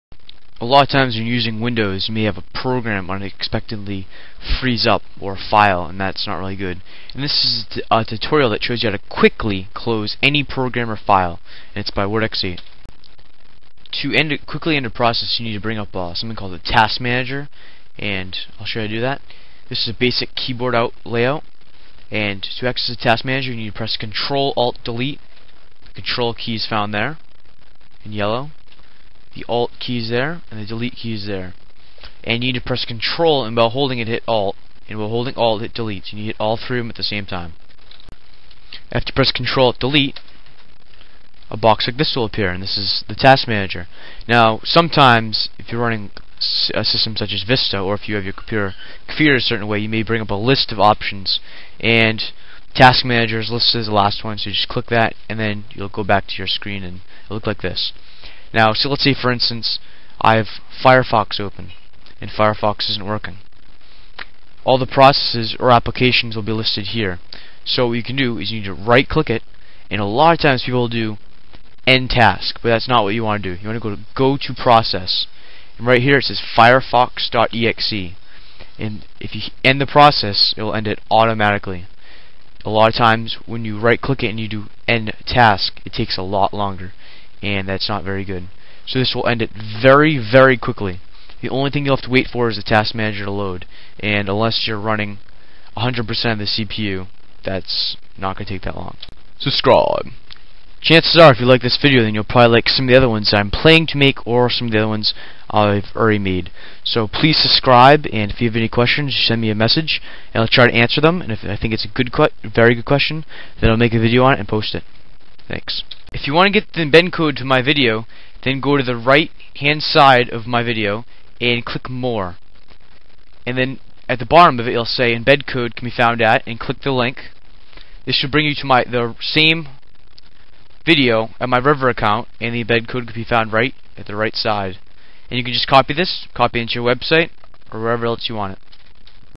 How to Quickly Close (Force Quit) Any Program
This is a tutorial that shows you how to force quit any program, whether or not it is an application, folder etc. To force quit something press ctrl+alt+delete. This will bring up the task manager. (or start the task manager from the list of options) Once task manager is open, right click the program you wish to close and select go-to process. Then press the end process button. Comment the video please.
~Wartex8